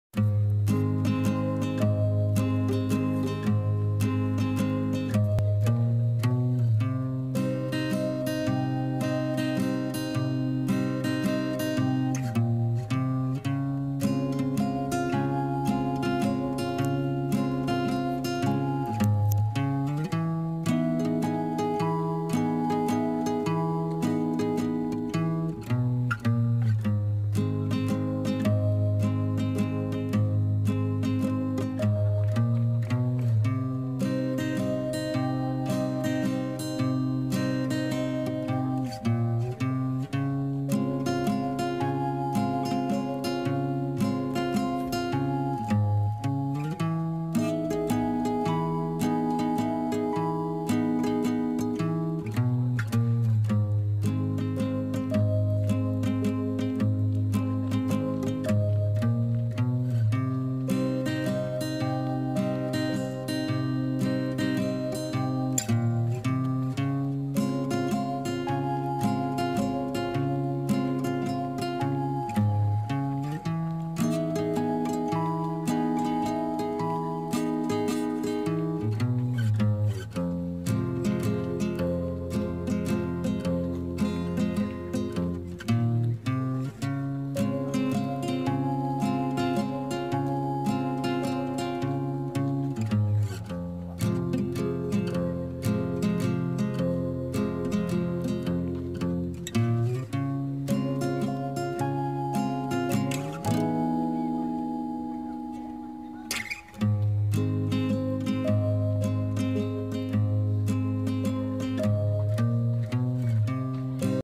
ブルームーンを見たくて夜桜見物に行って来たよ‼️‼️チャンネル登録してネ❤️❤️
とても素敵な夜桜でした。